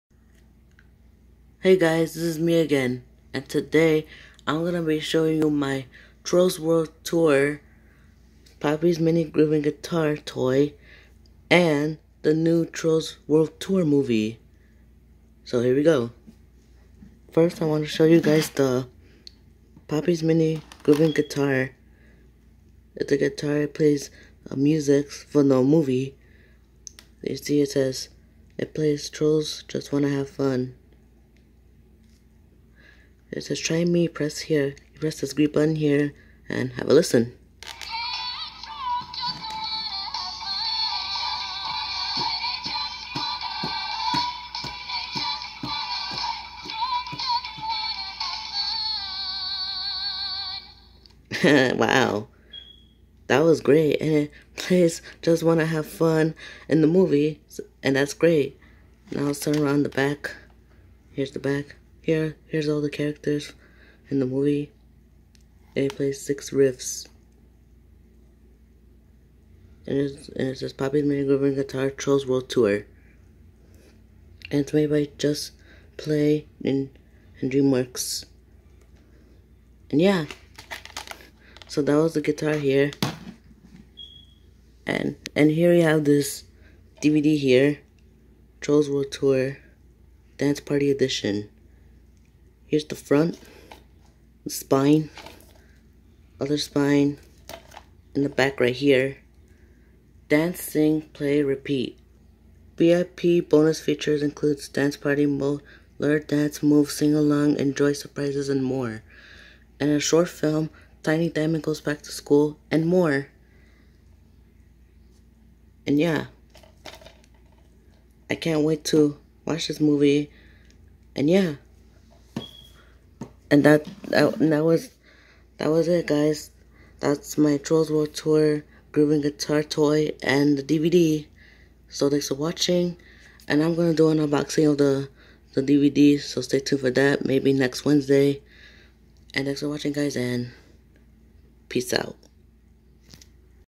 My Trolls World Tour Poppy's Mini grooving guitar Toy and Trolls world tour DVD!!!
The Trolls world tour movie just came out Today on DVD And Blu-ray!

(I got the DVD at Walmart yesterday!)